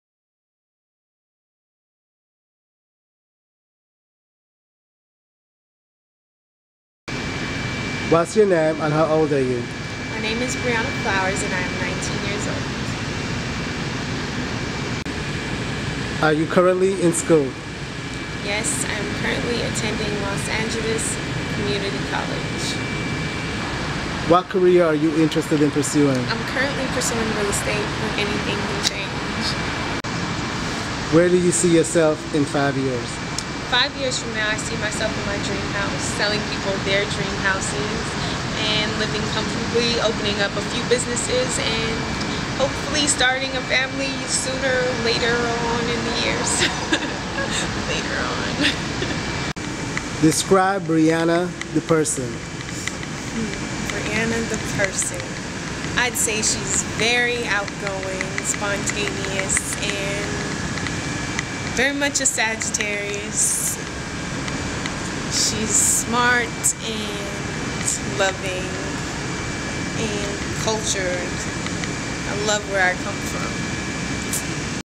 Briana Flowers
Representing Orange Walk in the Miss Belize California Beauty Pageant, to be held November 16 at Hollywood Park Casino